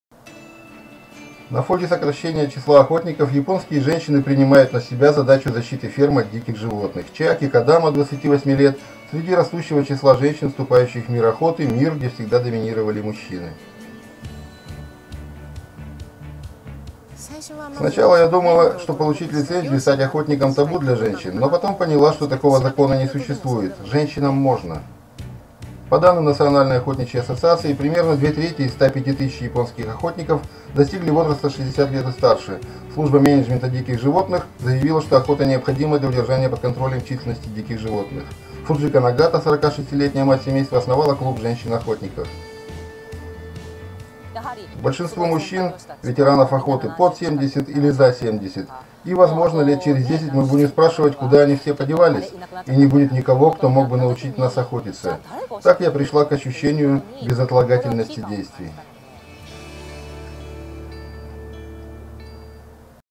Японские женщины вступают в мир охоты
С конца 1990-х годов поголовье оленей в Японии выросло с 400 тысяч до более чем 3 миллионов особей. Численность кабана за этот же период удвоилась, достигнув 1 миллиона.
Количество охотников в Японии сократилось с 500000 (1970-е гг.) до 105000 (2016 г.), две трети из них достигли возраста 60 и более лет.
На этом фоне растёт количество женщин-охотниц.

Заметка об этом (изложение статьи Рейтер) на странице “Охотоведение”:
- в Фейсбуке –

Перевод и озвучивание ролика: С.П.Матвейчук

Источник видео: BOOM. A Decline In Japanese Hunters Sees Women Taking Up Arms To Protect Farms. Опубликовано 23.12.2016. URL